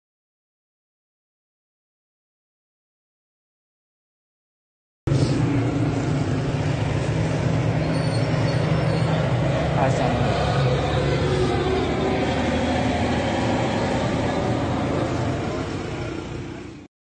Iran Protests - Authorities are still concerned over further protests in Behbahan city, Khuzestan
Iran Protests
Authorities are still concerned over further protests in Behbahan city, Khuzestan, despite passing a week from the latest protests on July 16.
"We don't want clerical regime," "No Gaza nor Lebanon, my life is for Iran," & "Death to Khamenei," chanted by citizens.